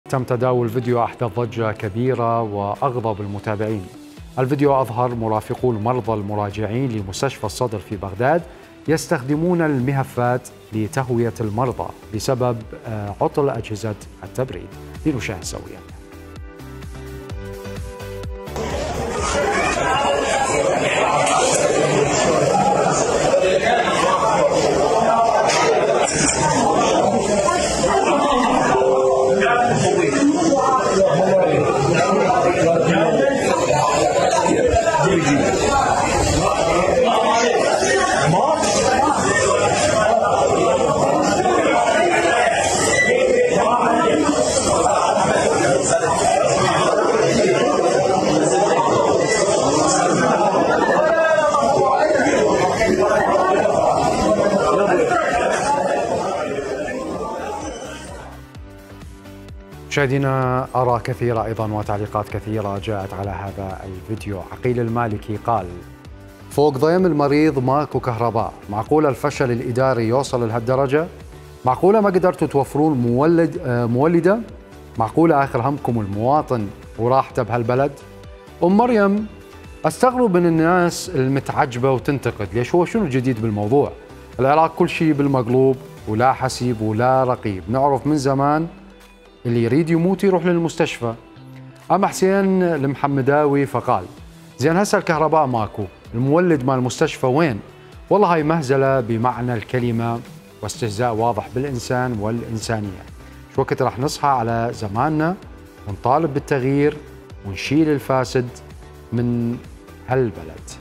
المهفات بديلا للتبريد في مستشفى الصدر ببغداد | UTrending#
التردد:

11354v HD
10727h SD